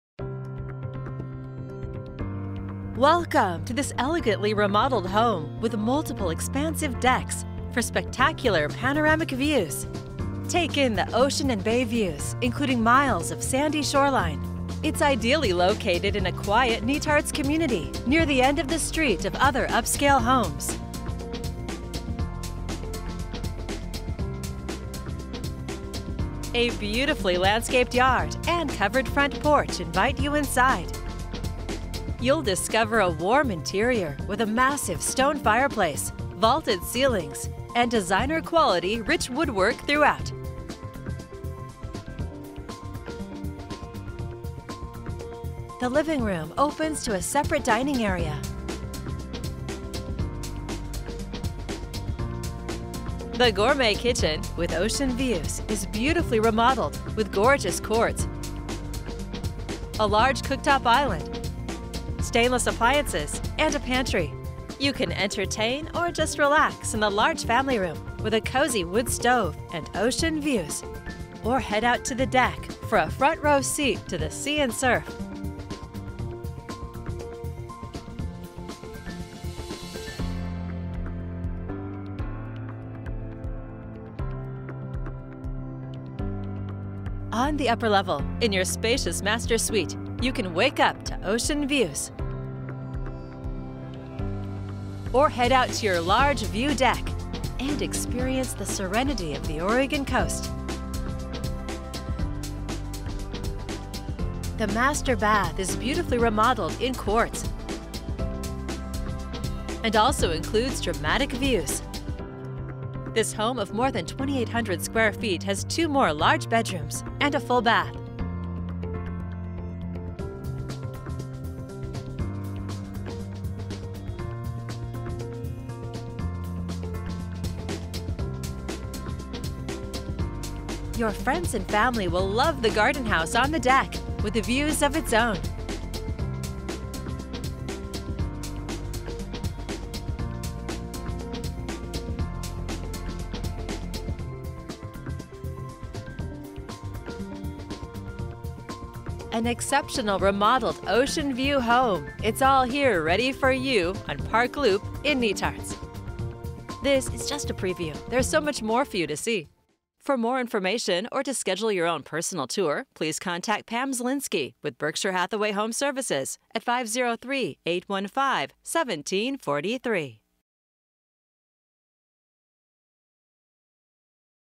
Spectacular Ocean Views ~ Remodeled Oregon Coast Home ~ Video of 1540 W Park Loop, Netarts
Heart pounding, spectacular panoramic ocean and bay views including miles of sandy shoreline. Elegantly remodeled kitchen and master suite. Designer quality, rich hardwoods, quartz. Garden house on deck. Massive stone fireplace + woodstove. Contact Pam Zielinski with Berkshire Hathaway HomeServices for more info.